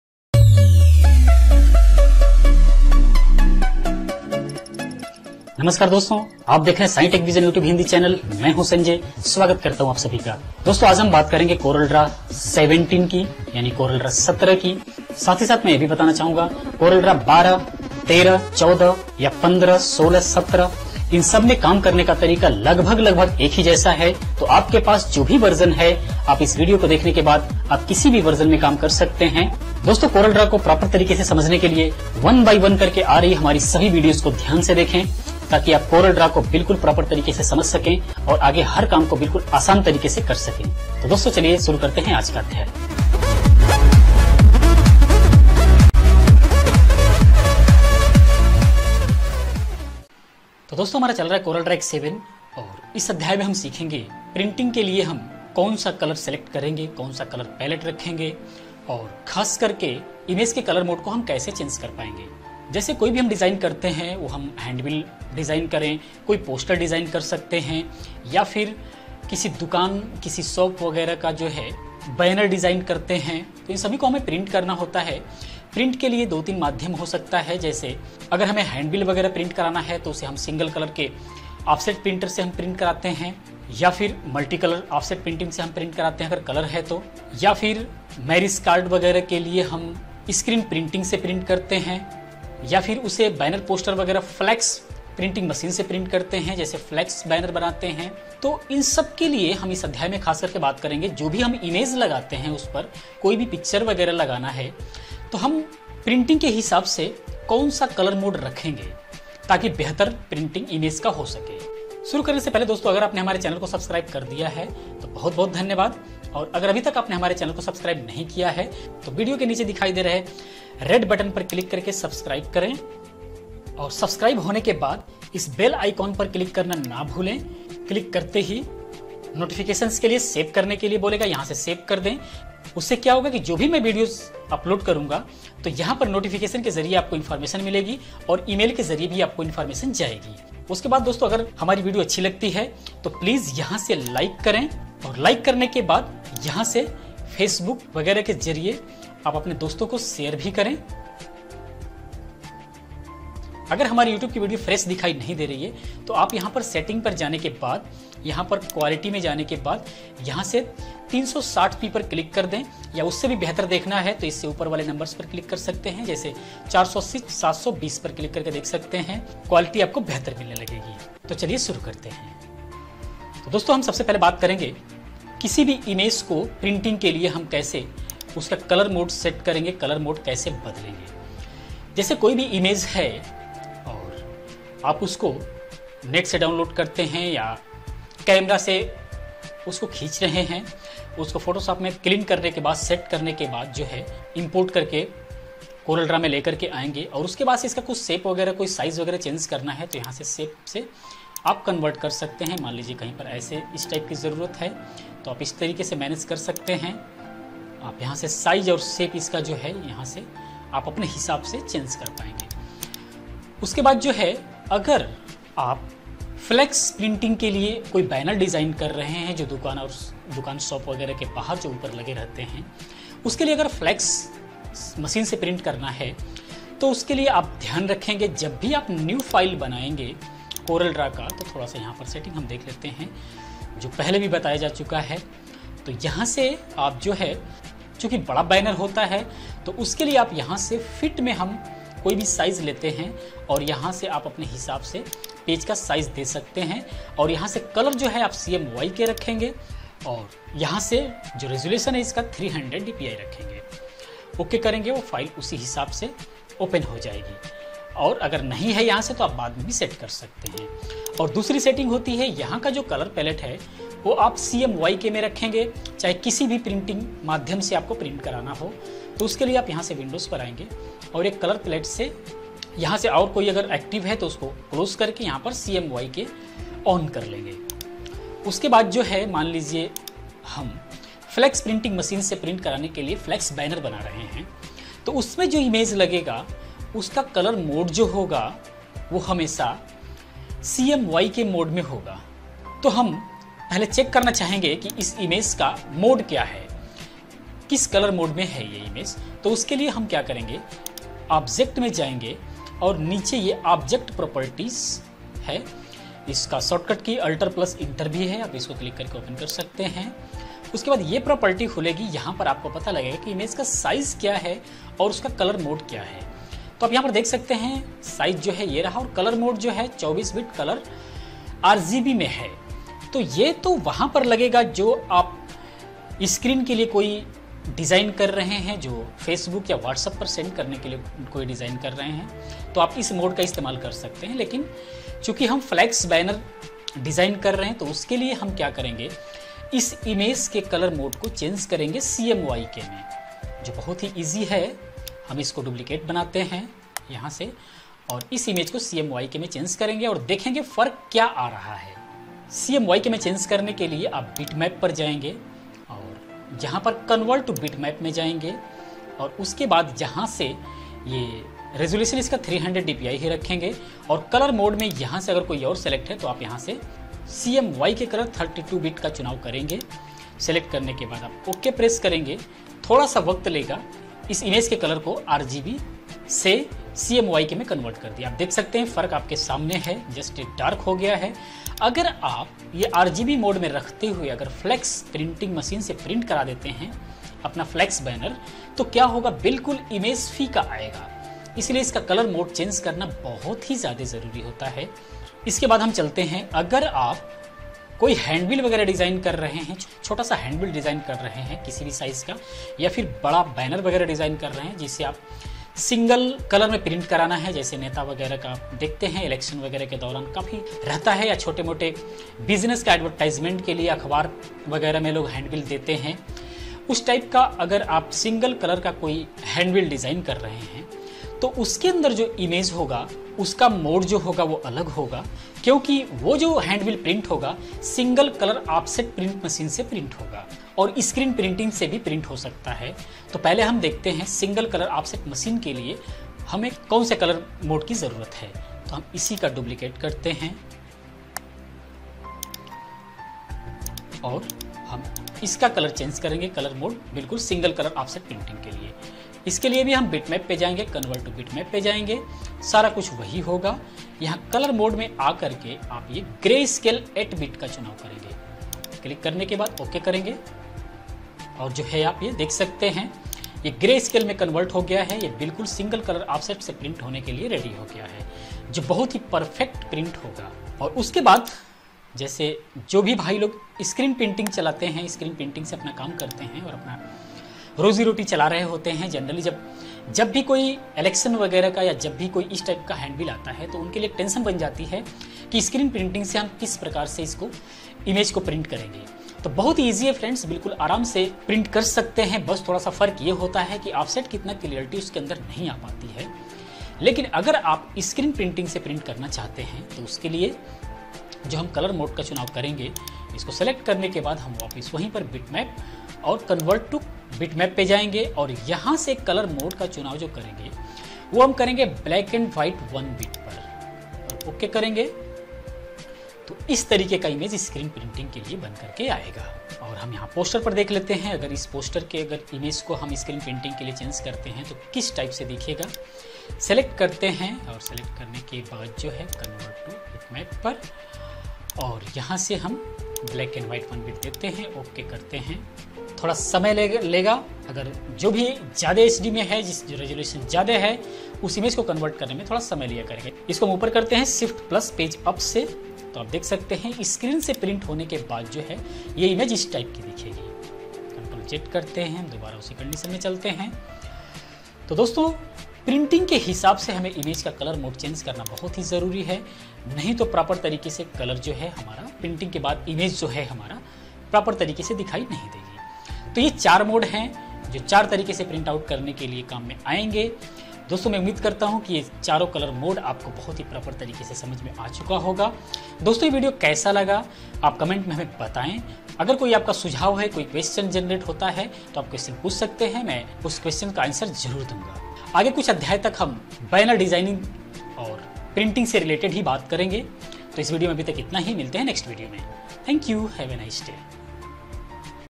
How to Change Image Color Mode for Printing in CorelDraw X-7 |Hindi|# 46
Namaskar Doston ! CorelDraw Ki is video me Printing ke liye Image ke Color Mode ko kaise change karenge aur kaun sa Color Mode Kaun se Printing ke liye sahi Hoga in sabhi ke bare me janenge. CorelDraw ko Achhe se Seekhane ke Liye  ek-ek Karke Aa Rahi Sabhi Videos Ko Dhyan Se Dekhen.